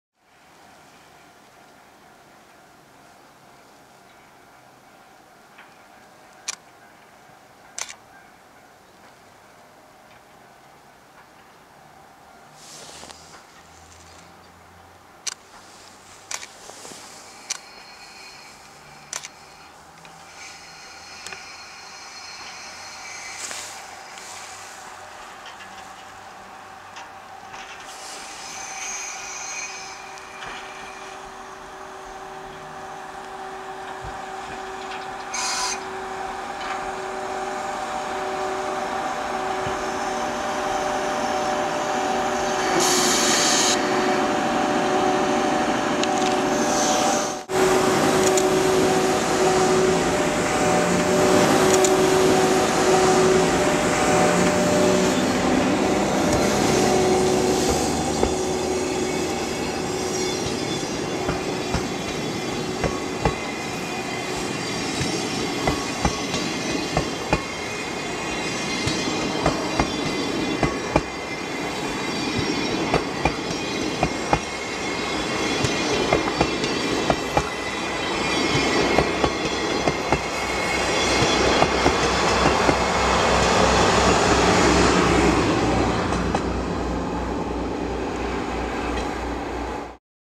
寝台特急「あけぼの」2022レ'10,2,13
夜の碇ヶ関駅です。EOS 5D Mark2 使用。iMovie'09にてエフェクト。